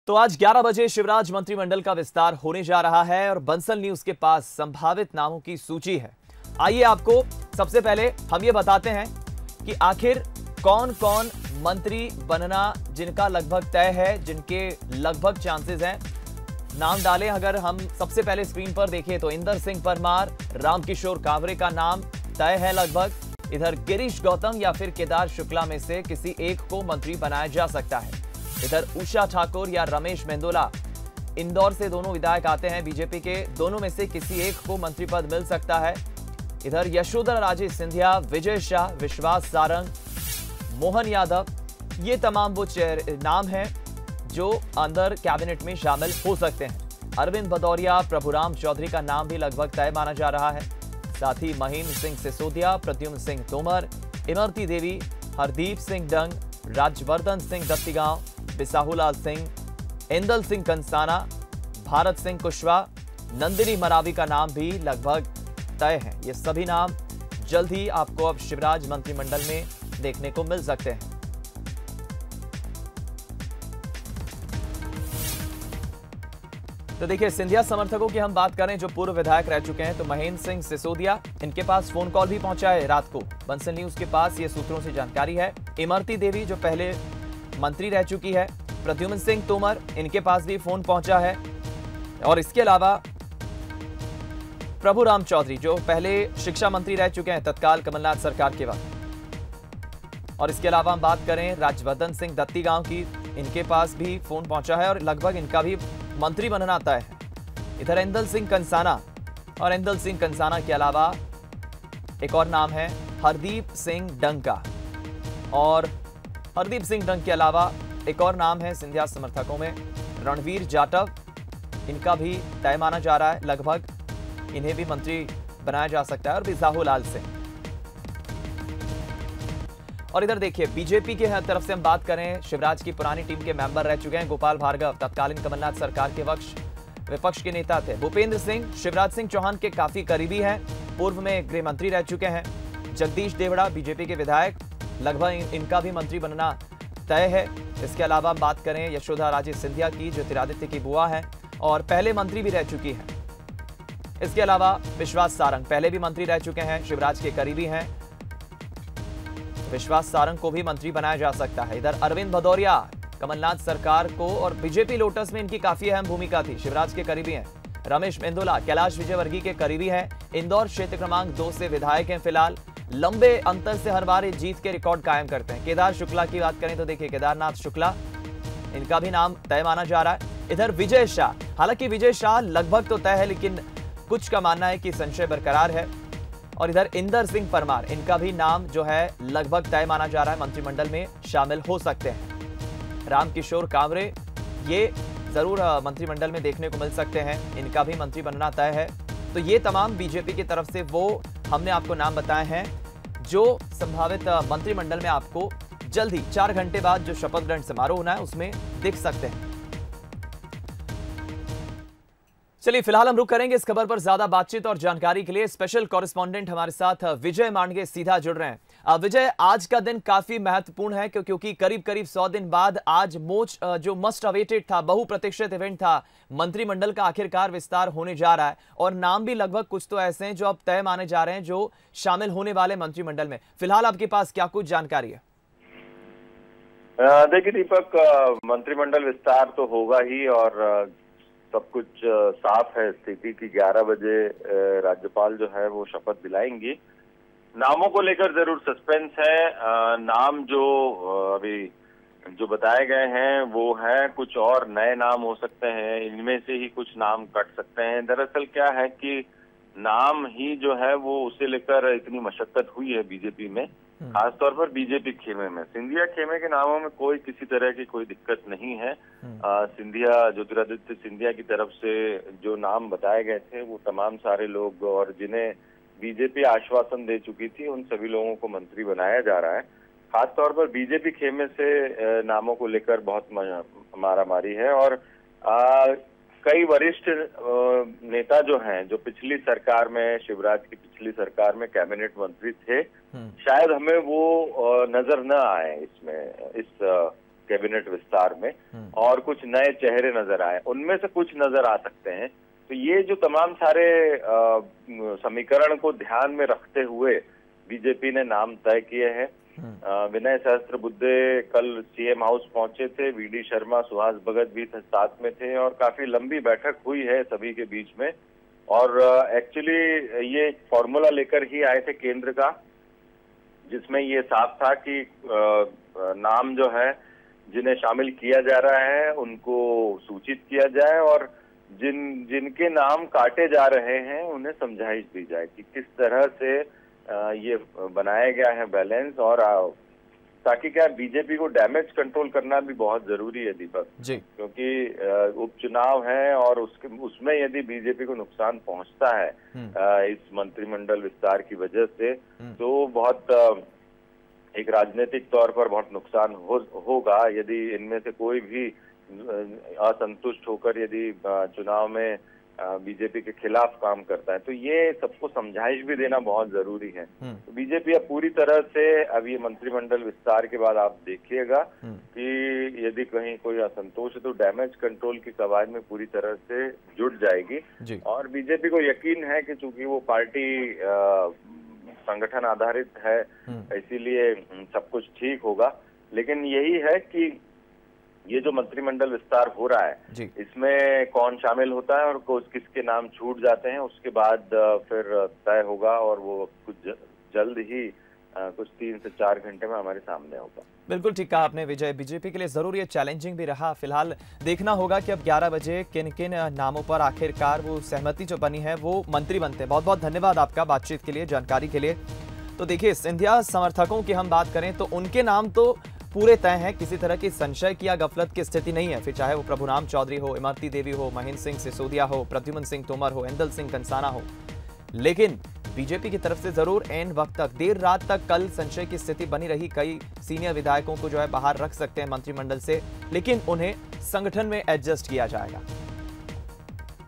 बड़ी खबर-मंत्रिमंडल विस्तार के संभावित नामों की सूची-अरविन्द भदौरिया , प्रभुराम चौधरी का नाम लगभग तय
बड़ी खबर-मंत्रिमंडल विस्तार के संभावित नामों की सूची - अरविन्द भदौरिया और प्रभुराम चौधरी का नाम लगभग तय